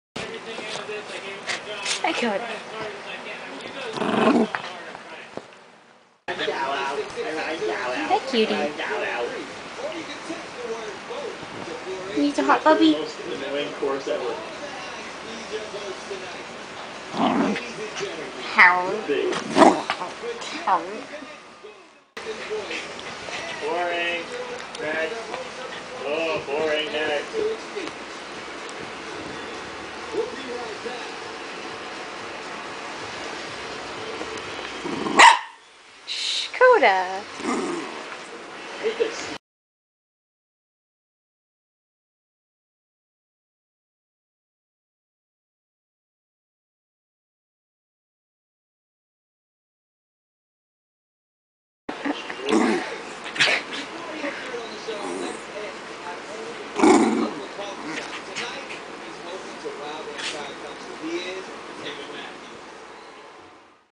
Koda Quirks: Attention Sponge
Koda Quirks! The cute things my Corgi, Koda, does ;D
Trying to capture all of the cute things that Koda does before she grows out of them
(Since she's starting to grow out of "yelling" at her food...)

This one isn't that special, I suppose...but it's cute!
Koda's an attention whore...need I say more?